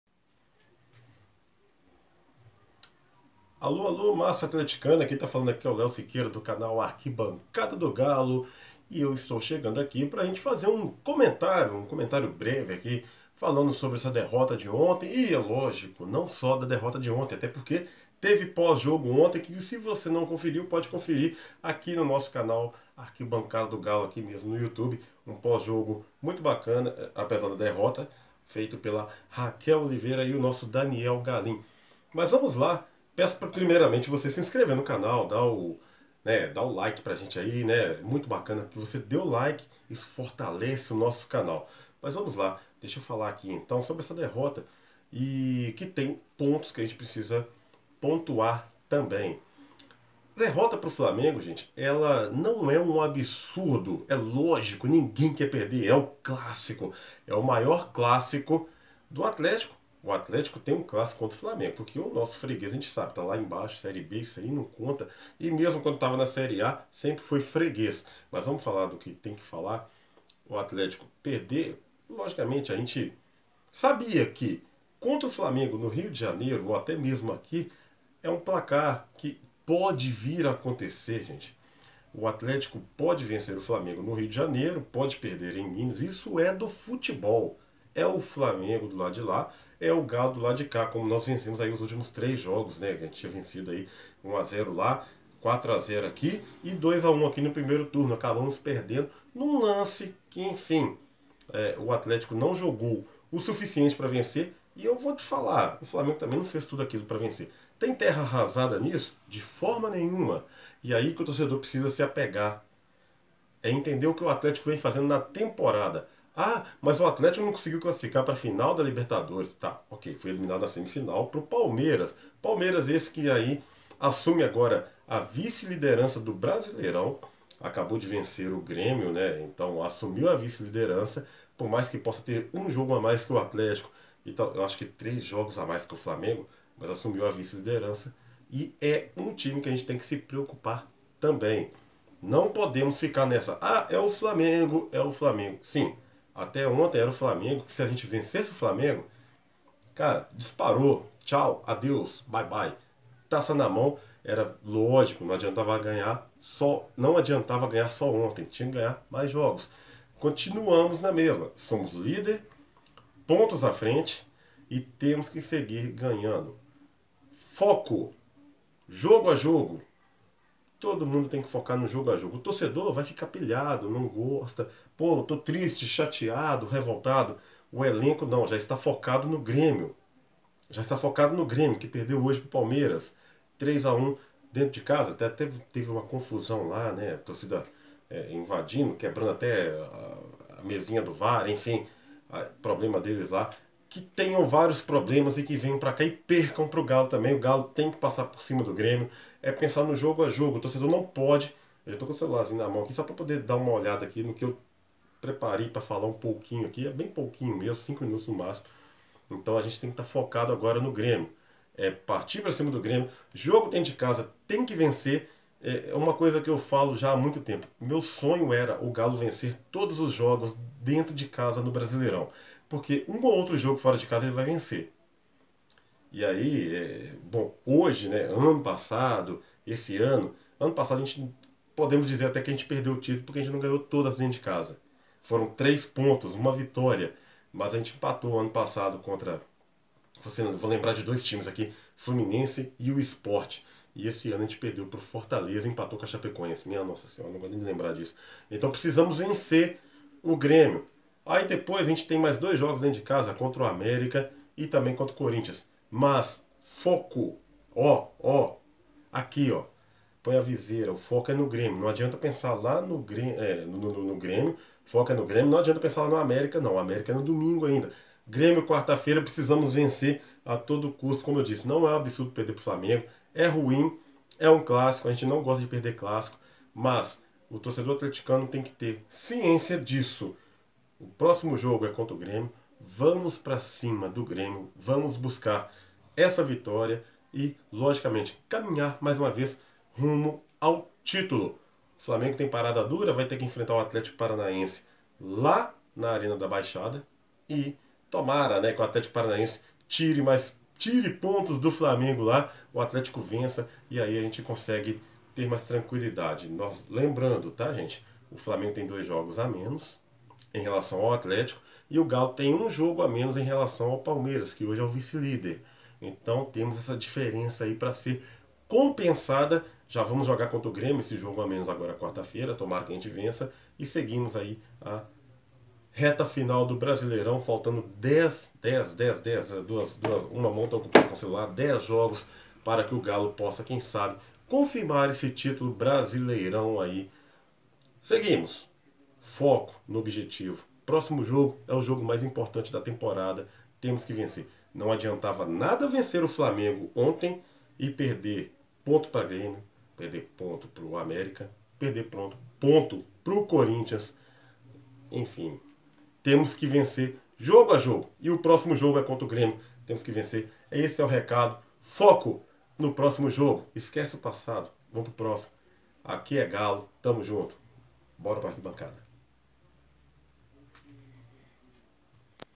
Sem terra arrasada, foco no próximo jogo. Vamu Galoooooo.
Sem lamentações, o foco é no próximo jogo.

É na arquibancada onde...
O pobre, o rico, o preto e o branco se juntam
Todos tem voz e a voz de todos tem um só tom
Todos falam, gritam e suas vozes ecoam como uma só
Todo mundo junto vive esse amor, esse modo de vida chamado Clube Atlético Mineiro.